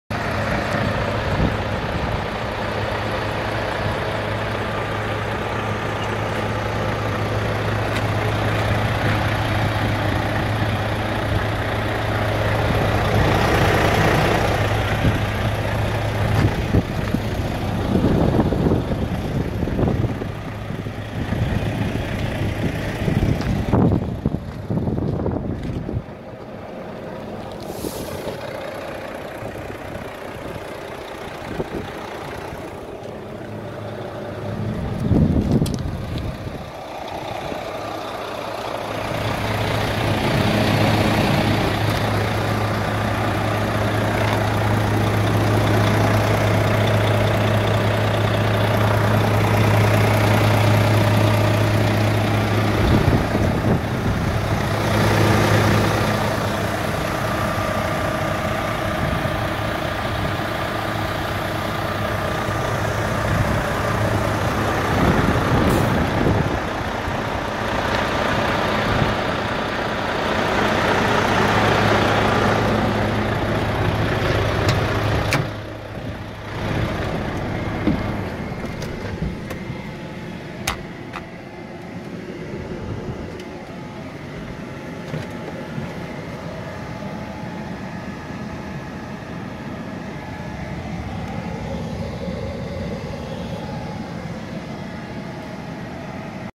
VOLVO FH500 GLOBETROTTER XL 6X2 TRACTOR UNIT 2012 SY12 CPE
Volvo 500Bhp Euro 5 Engine, VEB, I-Shift Semi Automatic Gearbox, Midlift Axle, Ecas Air Suspension, Sliding Fifth Wheel, Alloy Fuel Tank, 3.9M Wheelbase, Globetrotter XL High Roof Twin Sleeper Cab, Full Air Kit, Sunvisor, Electric Windows/Mirrors/Sunroof, Air Conditioning, Cruise Control.